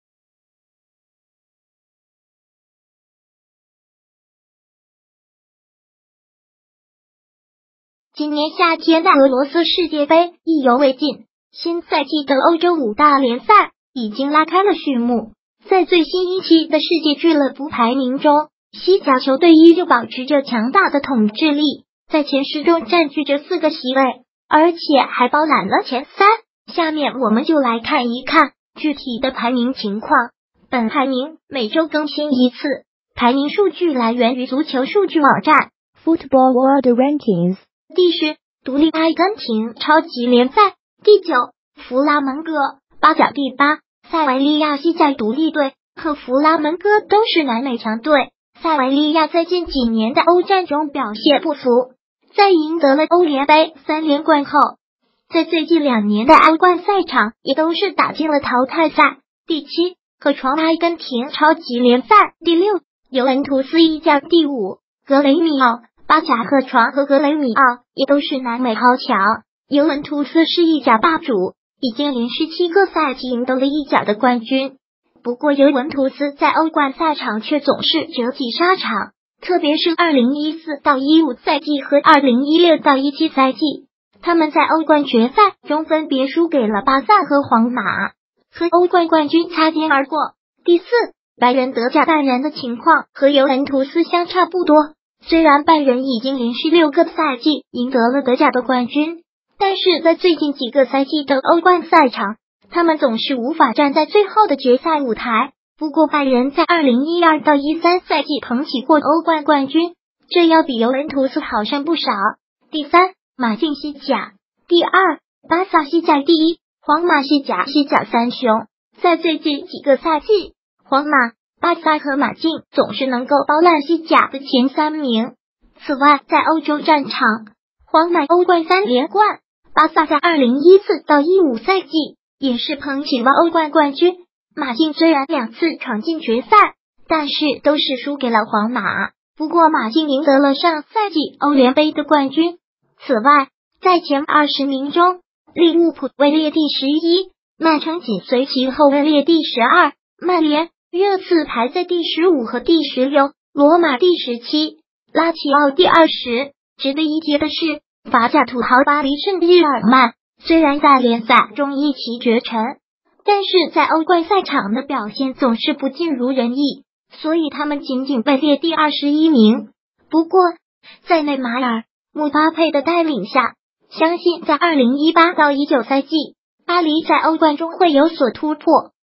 世界最新俱乐部排名，西甲前十占4席，利物浦力压曼城排第11
今年夏天的俄罗斯世界杯意犹未尽，新赛季的欧洲五大联赛已经拉开了序幕。在最新一期的世界俱乐部排名中，西甲球队依旧保持着强大的统治力，在前十中占据着4个席位，而且还包揽了前三，下面我们就来看一看具体的排名情况。（本排名每周更新一次，排名数据来源于足球数据网站：Football World Rankings）第十、独立 阿根廷超级联赛第九、弗拉门戈 巴甲第八、塞维利亚 西甲独立队和弗拉门戈都是南美强队，塞维利亚在近几年的欧战中表现不俗，在赢得了欧联杯三连冠后，在最近两年的欧冠赛场也都是打进了淘汰赛。第七、河床 阿根廷超级联赛第六、尤文图斯 意甲第五、格雷米奥 巴甲河床和格雷米奥也都是南美豪强，尤文图斯是意甲霸主，已经连续7个赛季赢得了意甲的冠军。不过尤文图斯在欧冠赛场却总是折戟沙场，特别是2014/15赛季和2016/17赛季，他们在欧冠决赛中分别输给了巴萨和皇马，和欧冠冠军擦肩而过。第四、拜仁 德甲拜仁的情况和尤文图斯相差不多，虽然拜仁已经连续6个赛季赢得了德甲的冠军，但是在最近几个赛季的欧冠赛场，他们总是无法站在最后的决赛舞台。不过拜仁在2012/13赛季捧起过欧冠冠军，这要比尤文图斯好